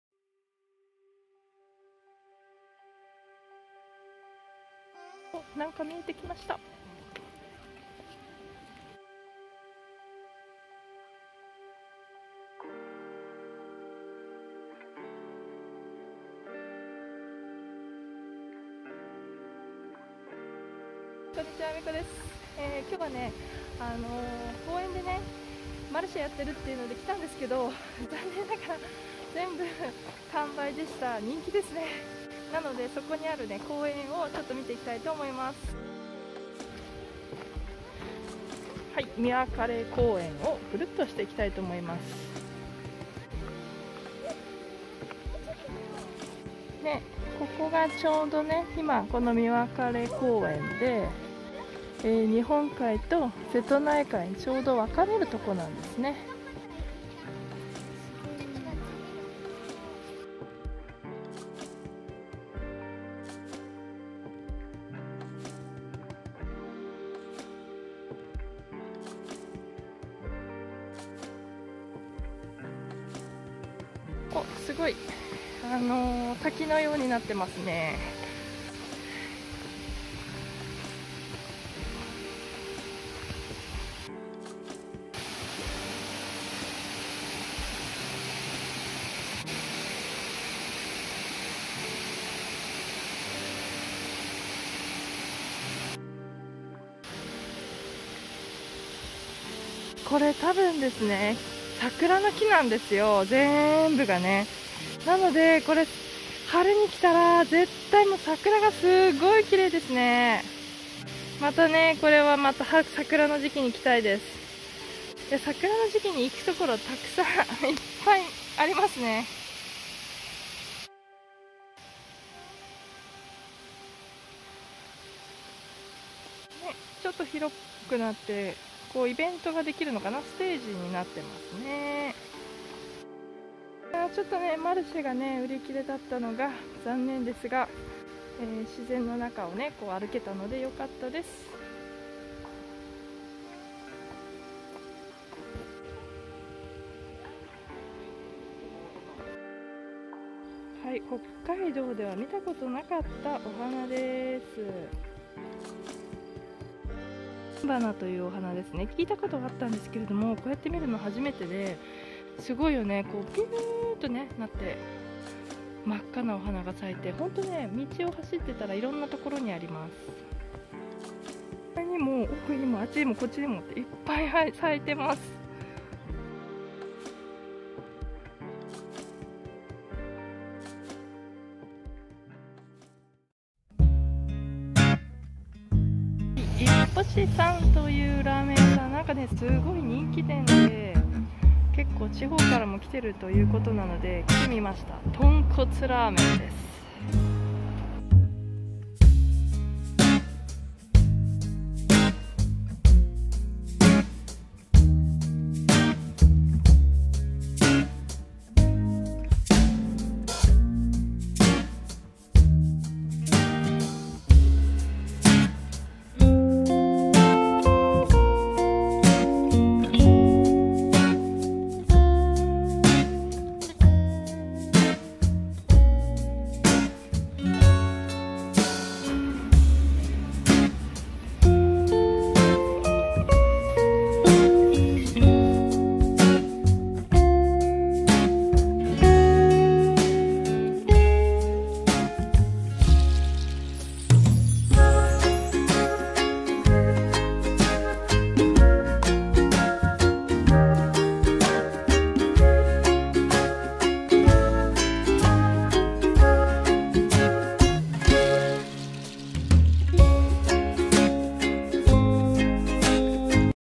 水分かれ公園といっぽしラーメンで丹波市の休日
兵庫県丹波市にある【水分れ公園】の紹介と
人気ラーメン店【いっぽし】さんの紹介です

のんびり過ごした一日の動画です(笑)
たまにはこんなのも😅